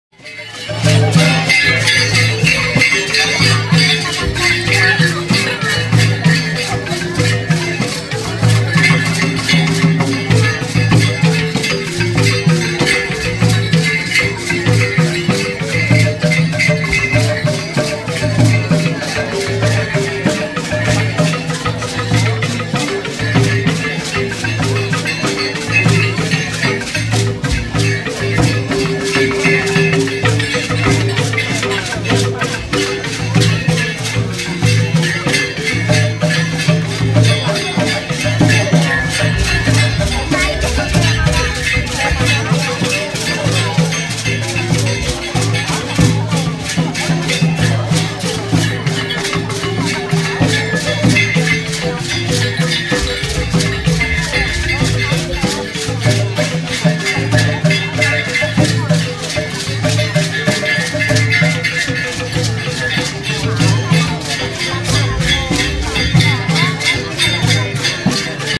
Tarian Gintur Dayak Deah Liyu
Pekan Budaya Tabalong Kalimantan Selatan, Feb 2016
Tarian Gintur Dayak Deah Liyu asal kabupaten Balangan. Gadis Dayak asli memakai pakaian kulit kayu.